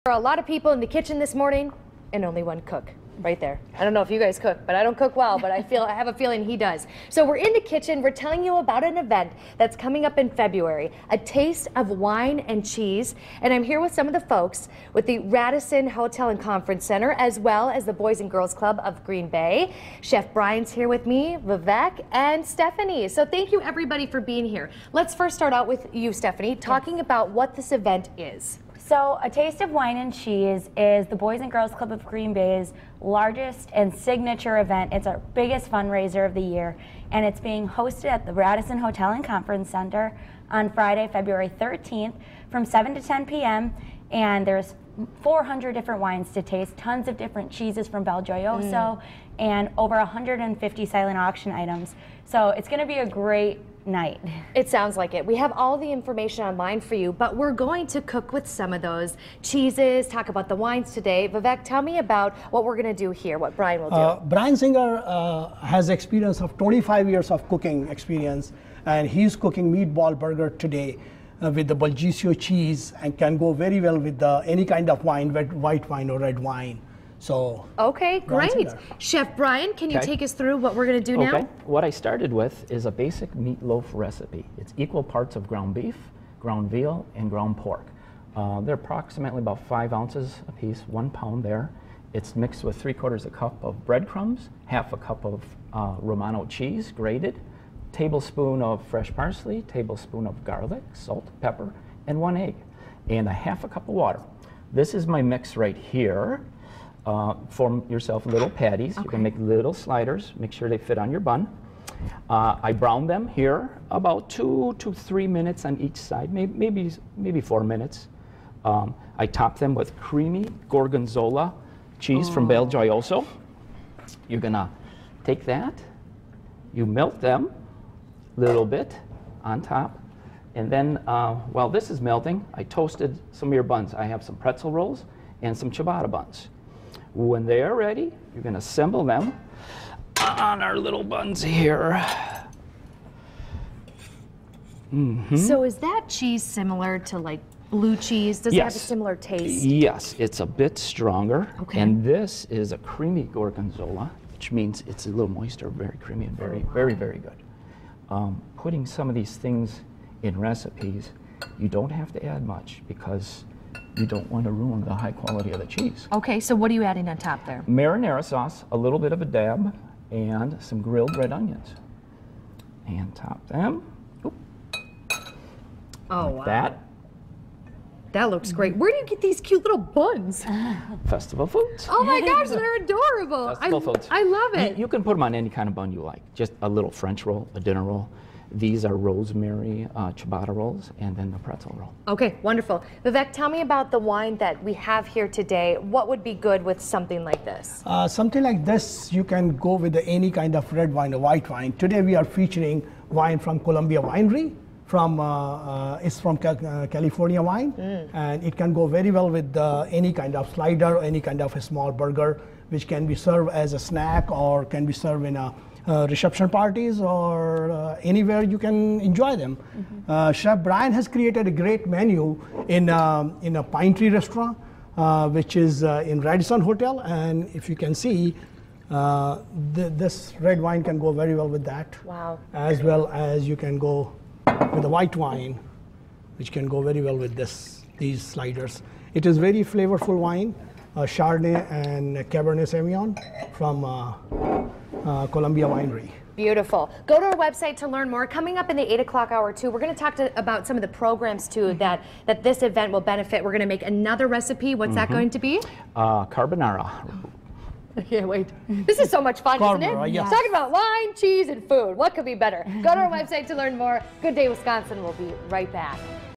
"A Taste of Wine and Cheese" event
Chef Brian created some delicious dishes this morning that will be at the event